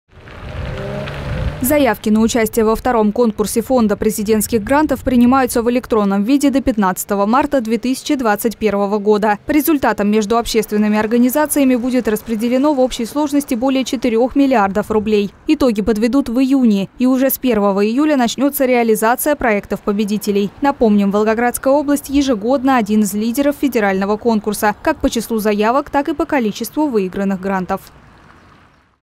В Волжском готовятся к конкурсу президентских грантов
Общественные организации города отправили более 60 социально-значимых проектов в сфере охраны здоровья, социальной поддержки, культуры, науки, экологии. Заявки на участие во втором конкурсе Фонда президентских грантов принимаются в электронном виде до 15 марта 2021 года.По результатам между общественными организациями будет распределено в общей сложности более 4 млрд рублей. Итоги подведут в июне. И уже с 1 июля начнется реализация проектов-победителей. Напомним, Волгоградская область ежегодно – один из лидеров федерального конкурса, как по числу заявок, так и по количеству выигранных грантов.